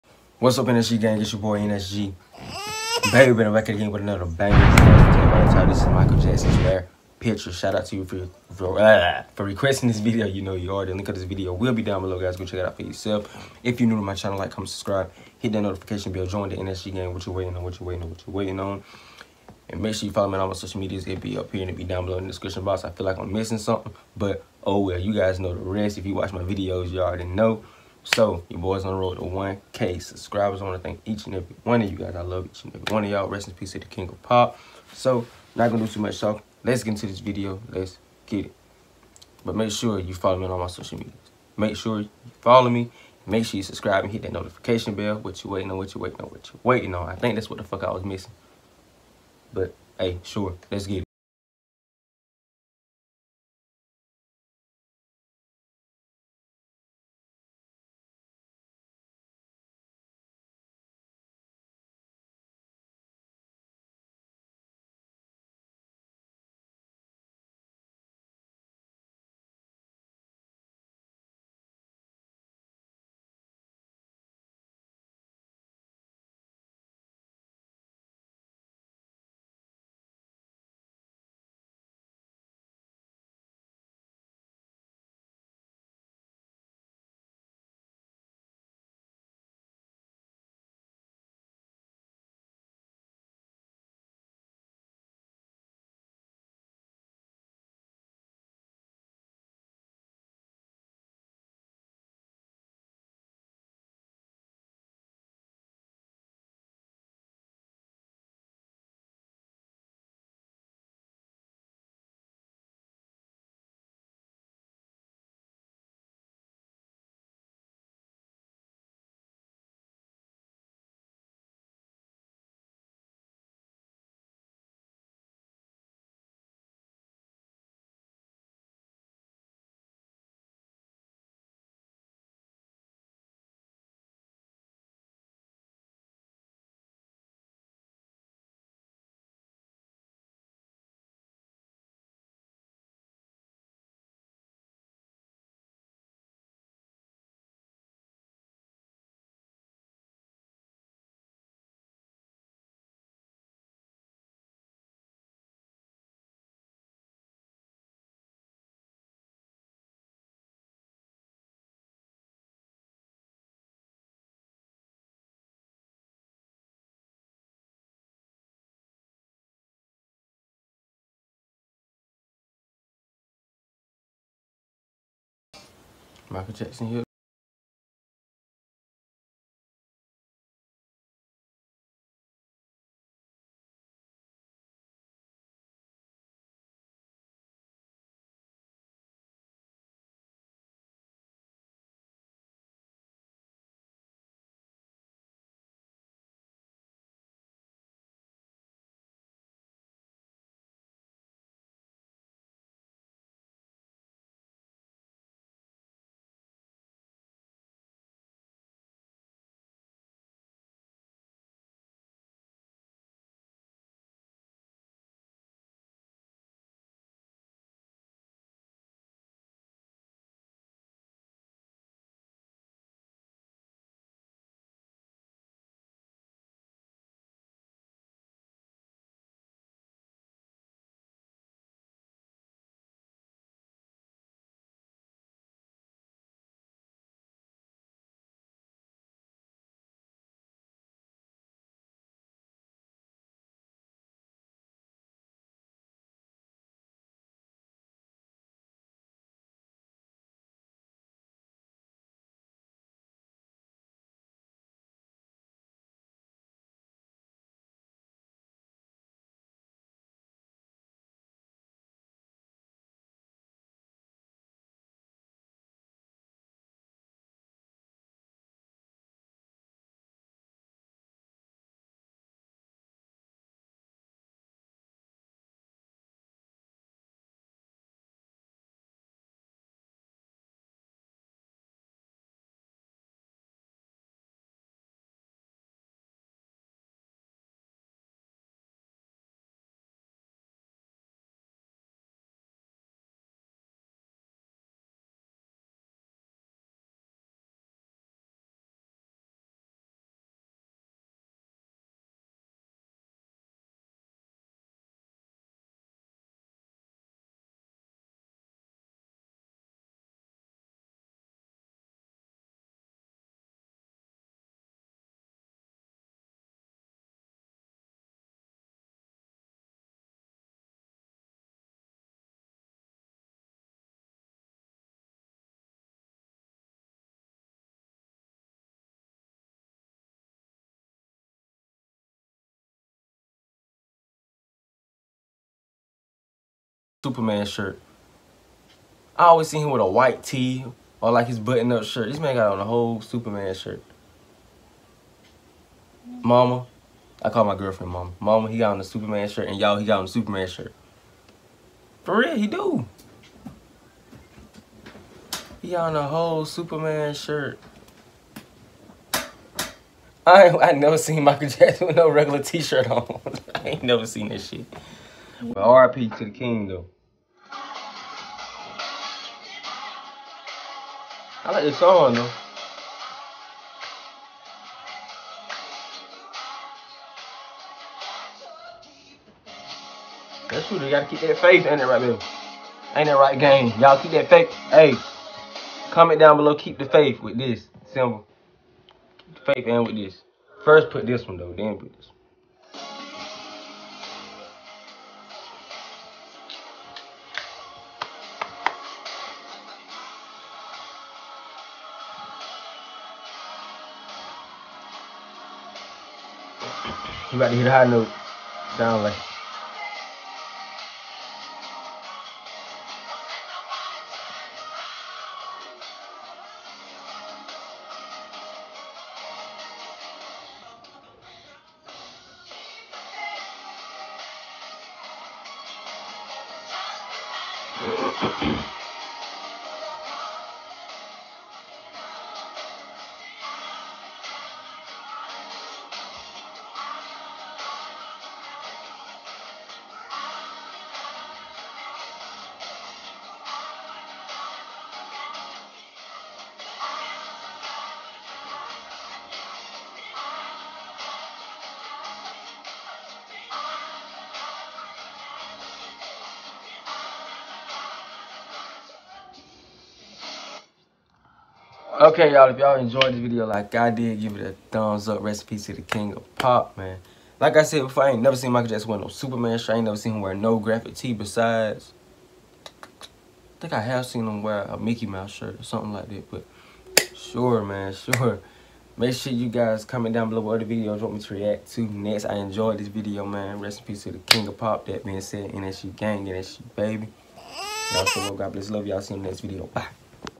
Michael Jackson Rare Pictures REACTION!!!!
IN TODAY’S VIDEO I WILL BE REACTING TO MICHAEL JACKSON RARE PICTURES